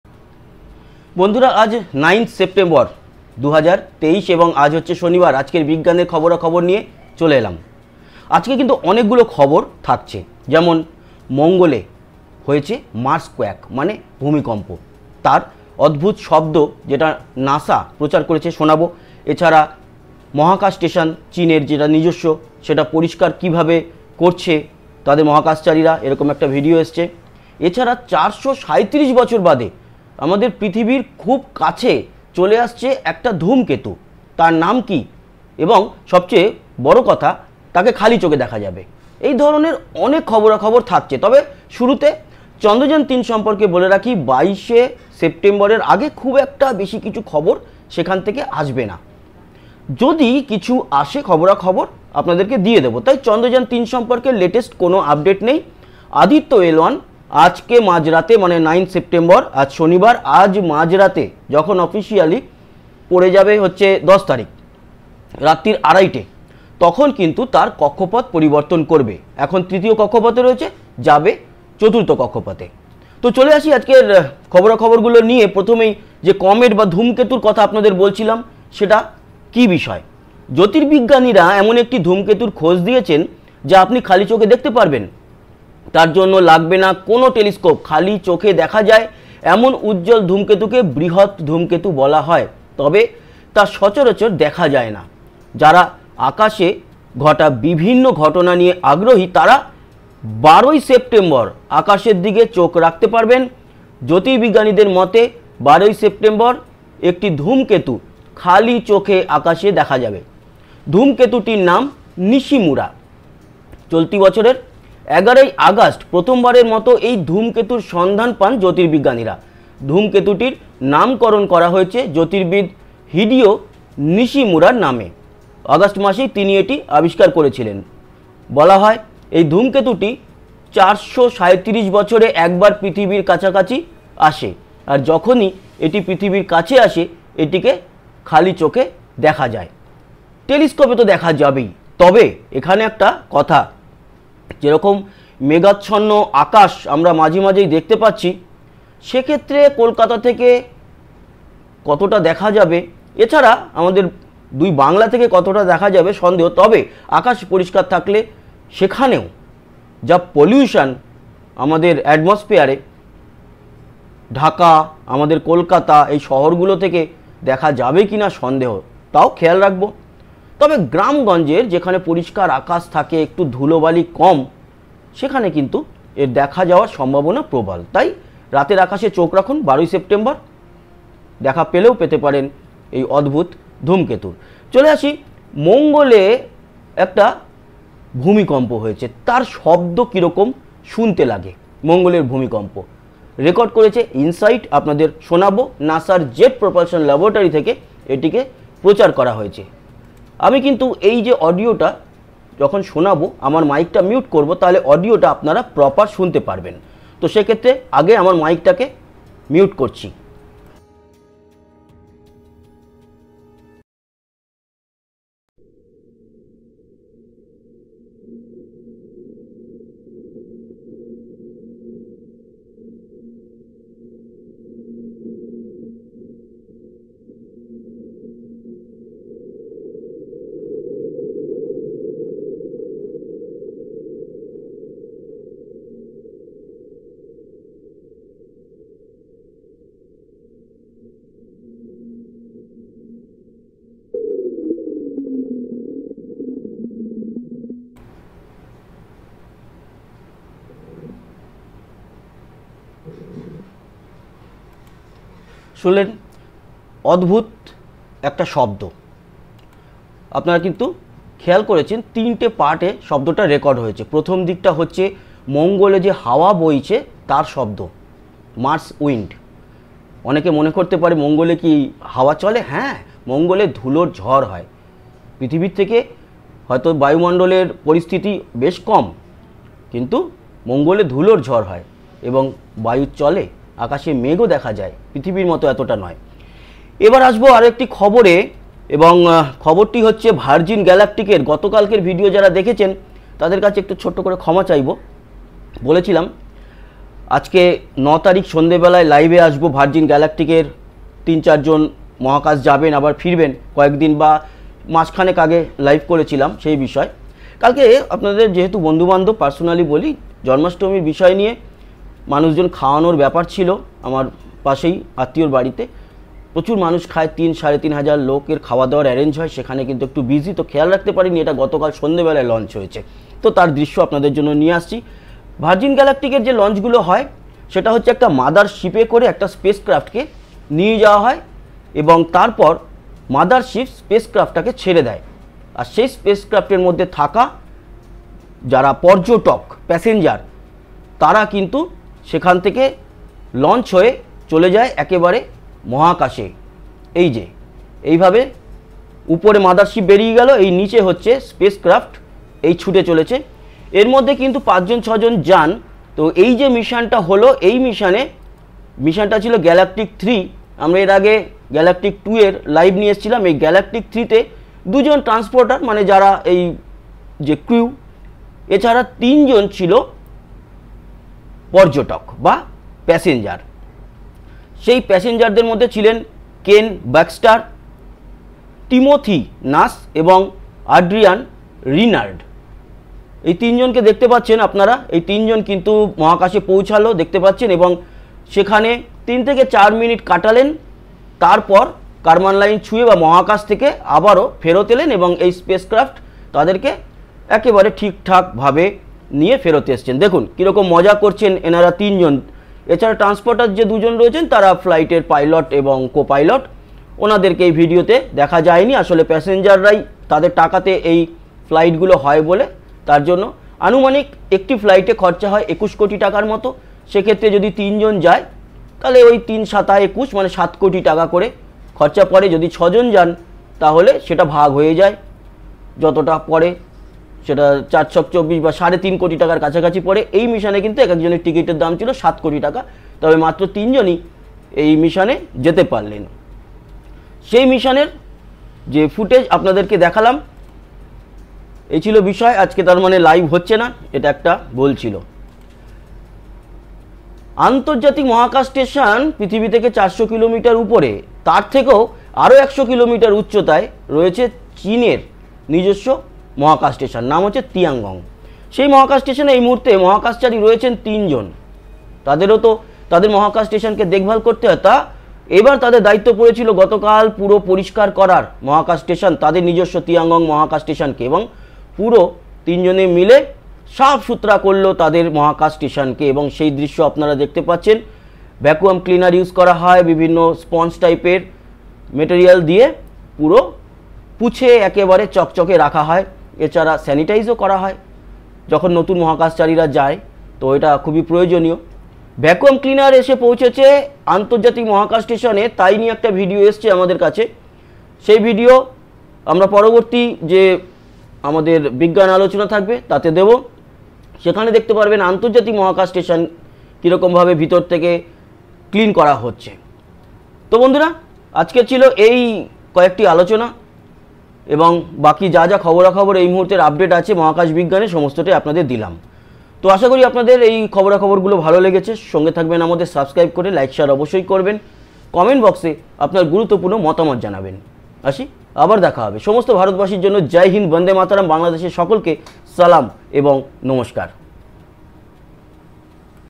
437 বছর পর পৃথিবীর সঙ্গে দেখা করতে আসছে নিশিমুরা ধূমকেতু, মঙ্গলে ভূমিকম্প, Chandrayaan 3, Space News
Galactic 03 - Virgin Galactic’s third commercial spaceflight – was launched from Spaceport America, New Mexico, on 8 September 2023, at 08:34 local time. VSS Unity, the second SpaceShipTwo, transported three private passengers: Ken Baxter, Timothy Nash and Adrian Reynard; as well as Beth Moses (Chief Astronaut Instructor). The flight reached an apogee of 55 miles (88,5 km).